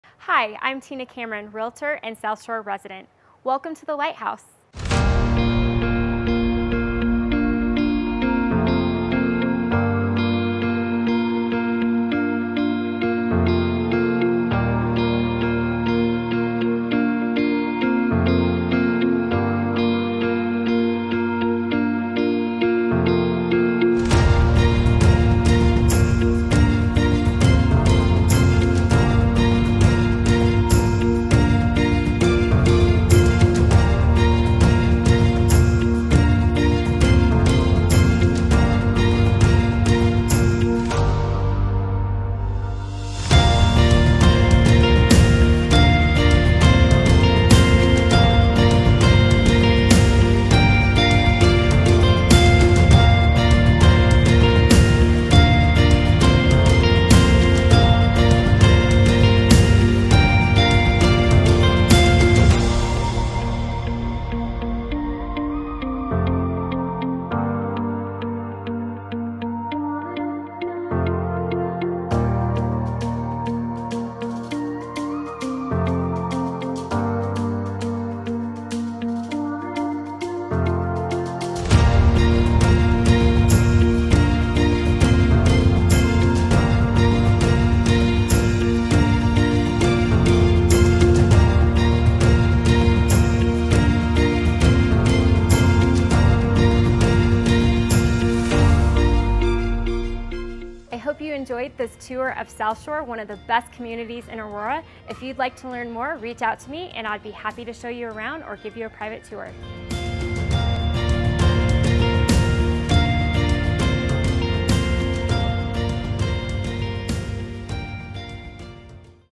The Lighthouse at Southshore
Located in the premier lake community of Southshore, the Lighthouse is one of two stunning clubhouses in Southshore. Enjoy fitness classes, a gym workout, the game room, an array of vibrant Southshore community events, and even complimentary beer/wine on tap for residents. This is the place to be!

I am a local Realtor and area expert and I'd be happy to help.